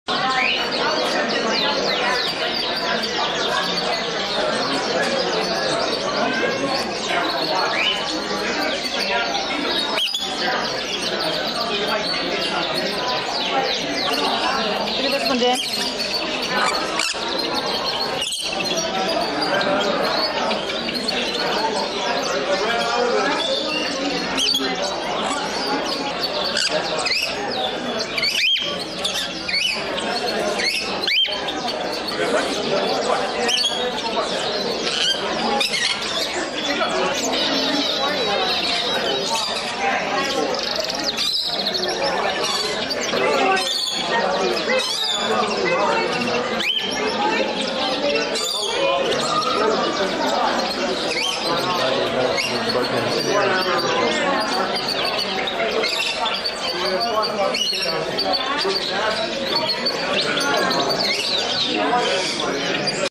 Lorikeet, Sun Conure, Rosella's, Budgies and more at the Bird Show 2012
These are some birds I saw at the bird show. I saw so many of them, watch for the other videos of more birds at the show.

🔸Social Media 🔸